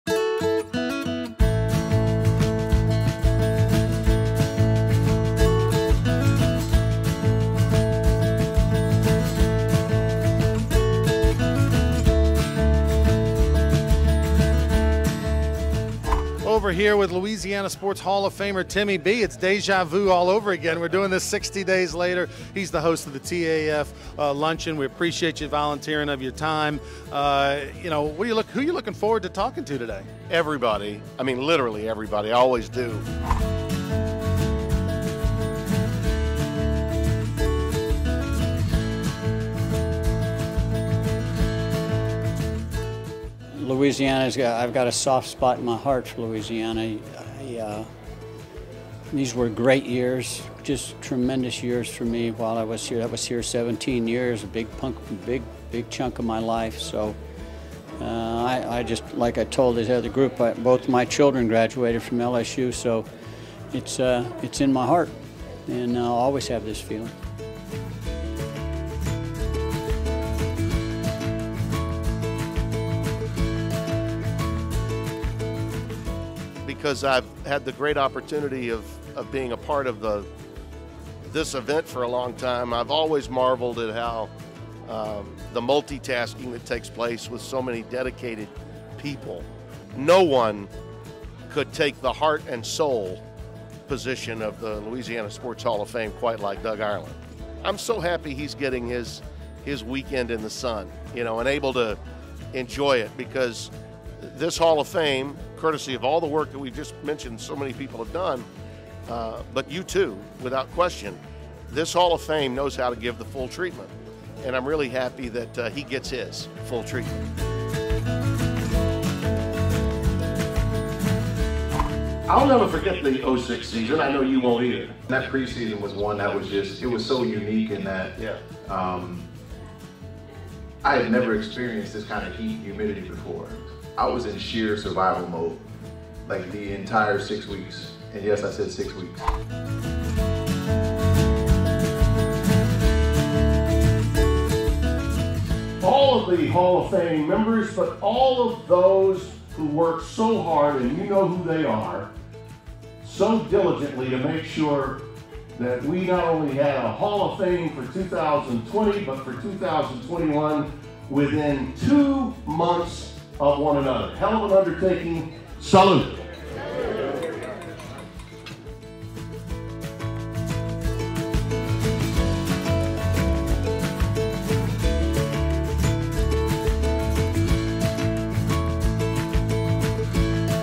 August 28, 2021 - LSHOF 2021 Induction Class Roundtable Luncheon presented by TAF
Tim Brando hosts a roundtable luncheon for the 2021 Louisiana Sports Hall of Fame induction class